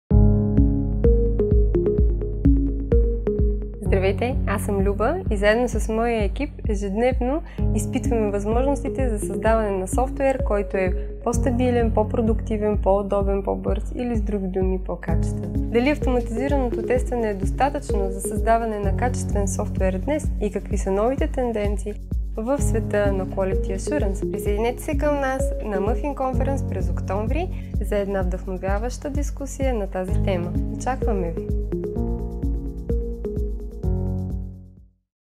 QA Trends Discussion Panel by Muffin Conference 2017
Our QA Masters are creating a better quality software on daily basis. Join us on MUFFIN Conference 2017 Sofia to discuss whether automated testing is enough and what are the QA trends.